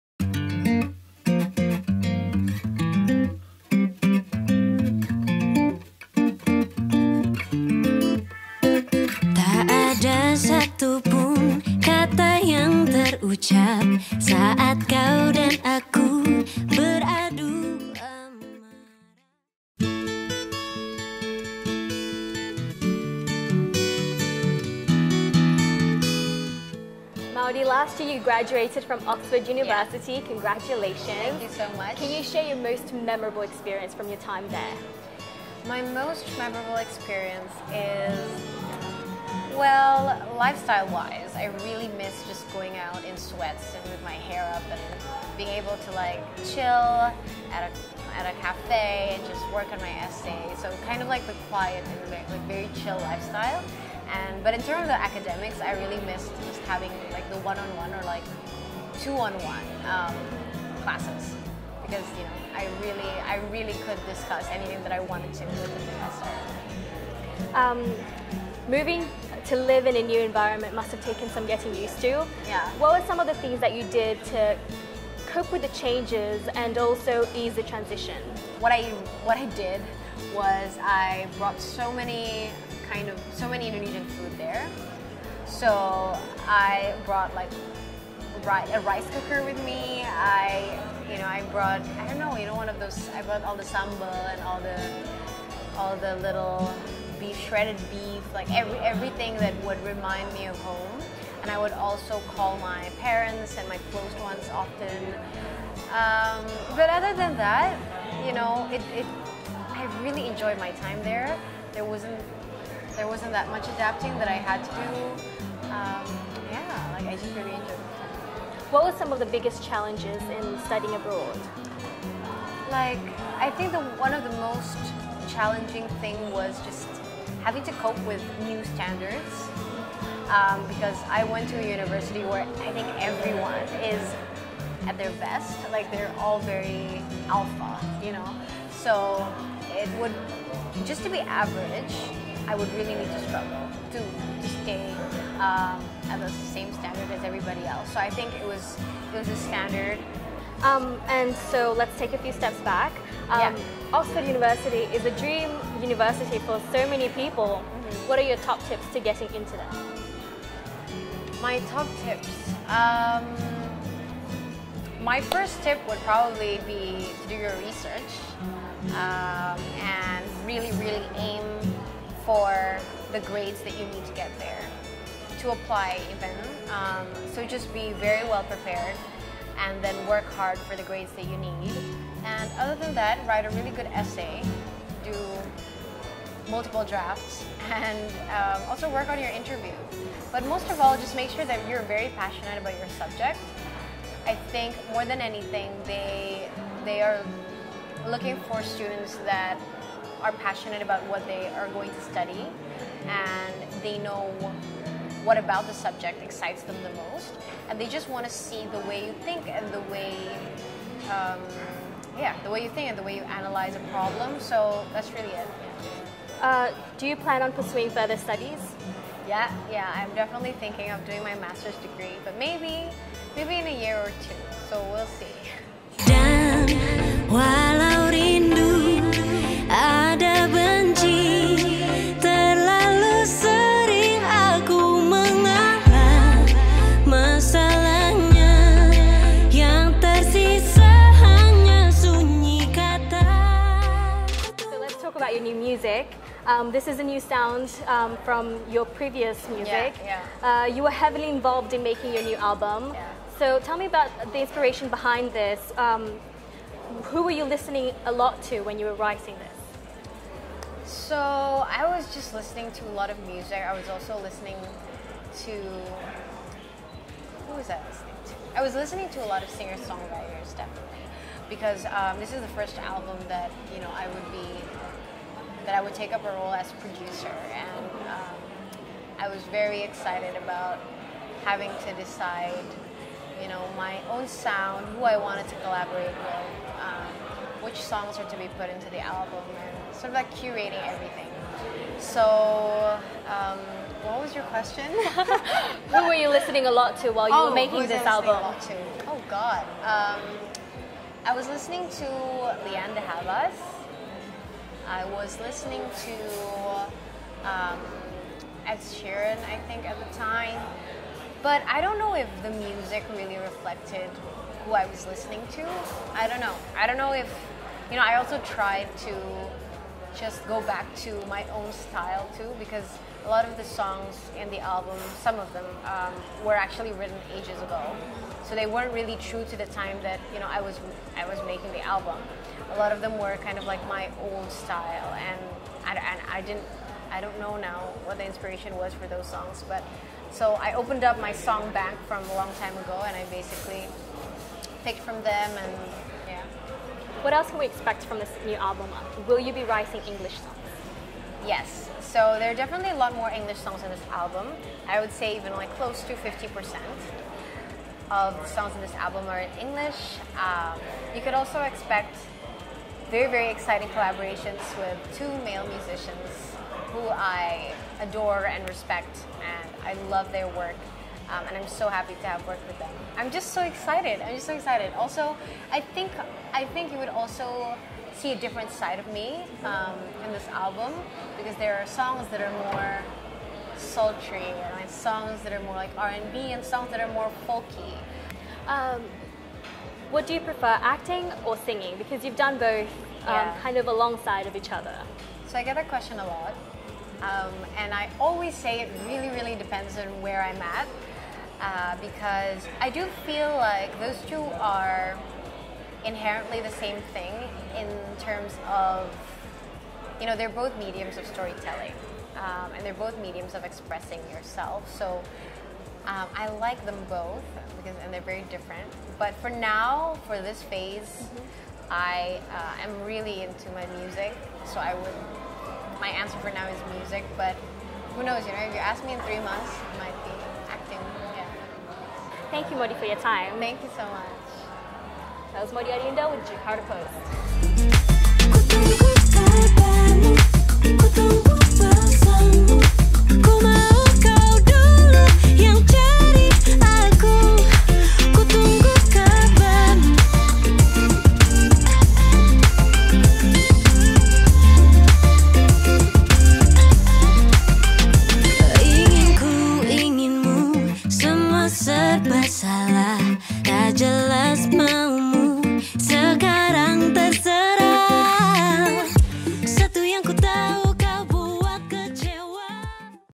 Maudy Ayunda after Oxford, before new album
A degree from one of the world’s most prestigious universities under her belt, movie roles, music, as well as being a beauty ambassador and a spokesperson for social movements, it seems nothing can stop Maudy Ayunda. (The Jakarta Post/ Liza Yosephine)